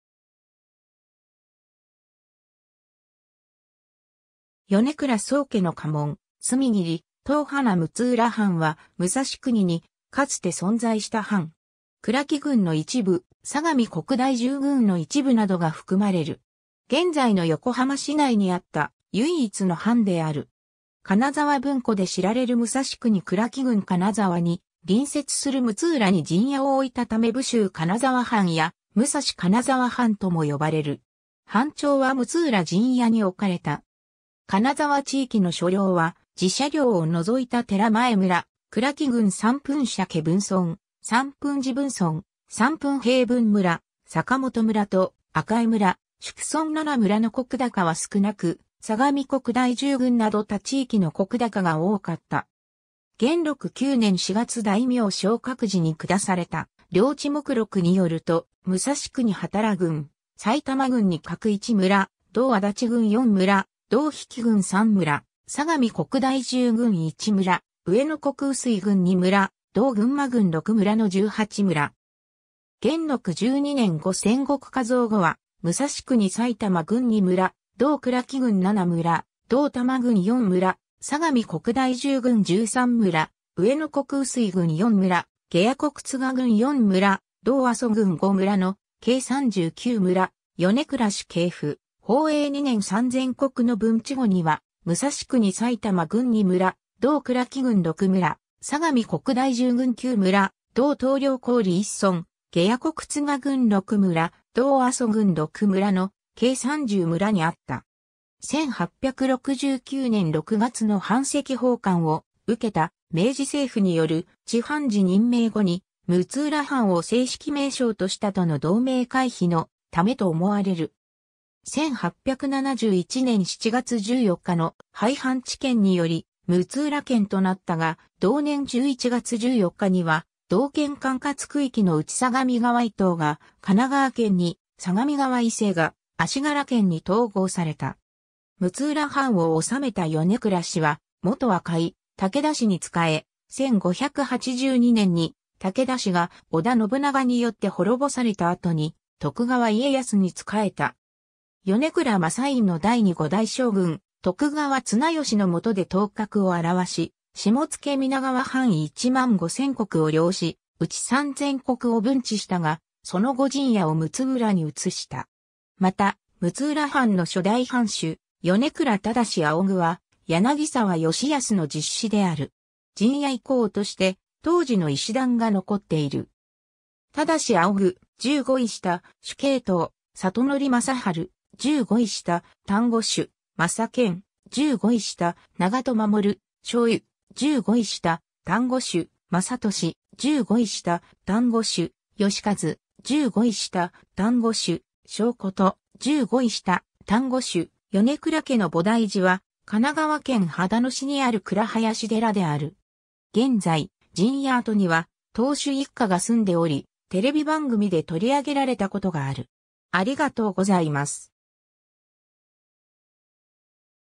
六浦藩
米倉宗家の家紋「隅切り唐花」 六浦藩（むつうらはん）は、武蔵国にかつて存在した藩。
久良岐郡の一部（現在の神奈川県横浜市金沢区）、相模国大住郡の一部（現在の神奈川県平塚市・秦野市）などが含まれる。
現在の横浜市内にあった唯一の藩である。
 金沢文庫で知られる武蔵国久良岐郡金沢（かねさわ、かなざわ）に隣接する六浦に陣屋を置いたため武州金沢藩や武蔵金沢藩とも呼ばれる。
藩庁は六浦陣屋に置かれた。
金沢地域の所領は寺社領を除いた寺前村、久良岐郡(六浦)三分社家分村、三分寺分村、三分平分村、坂本村と赤井村、宿村7村の石高は少なく、相模国大住郡など他地域の石高が多かった。
 元禄９年(1696)４月大名昇格時に下された「領知目録」(前掲書所収)によると、武蔵国幡羅郡・埼玉郡に各１村、同足立郡４村、同比企郡３村、相模国大住郡１村、上野国碓氷郡２村、同群馬郡６村の18村。
 元禄12年(1699)５千石加増後は、武蔵国埼玉郡２村、同久良岐郡７村、同多摩郡４村、相模国大住郡13村、上野国碓氷郡４村、下野国都賀郡４村、同安蘇郡５村の計39村「米倉氏系譜」、 宝永二年(1705)３千石の分知後には、武蔵国埼玉郡二村、同久良岐郡六村、相模国大住郡九村、同淘綾郡一村、下野国都賀郡６村、同安蘇郡６村の計30村にあった(前同)。
 1869年（明治2年）6月の版籍奉還を受けた明治政府による知藩事任命後に六浦藩を正式名称とした（加賀金沢藩（加賀藩）との同名回避のためと思われる）。
1871年（明治4年）7月14日の廃藩置県により六浦県となったが、同年11月14日には同県管轄区域のうち相模川以東が神奈川県に、相模川以西が足柄県に統合された。
 六浦藩を治めた米倉氏は、もとは甲斐武田氏に仕え、1582年（天正10年）に武田氏が織田信長によって滅ぼされた後に徳川家康に仕えた。
米倉昌尹の代に5代将軍徳川綱吉のもとで頭角をあらわし、下野皆川藩1万5000石を領し、うち3000石を分地したが、その後陣屋を六浦に移した。
また、六浦藩の初代藩主・米倉忠仰は柳沢吉保の実子である。
 陣屋遺構として、当時の石段が残っている。
 忠仰（ただすけ）〔従五位下・主計頭〕 里矩（さとなり） 昌晴（まさはる）〔従五位下・丹後守〕 昌賢（まさかた）〔従五位下・長門守〕 昌由（まさよし）〔従五位下・丹後守〕 昌俊（まさのり）〔従五位下・丹後守〕 昌寿（まさなが）〔従五位下・丹後守〕 昌言（まさこと）〔従五位下・丹後守〕 ※米倉家の菩提寺は、神奈川県秦野市にある蔵林寺である。
 現在、陣屋跡には当主一家が住んでおり、テレビ番組で取り上げられたことがある。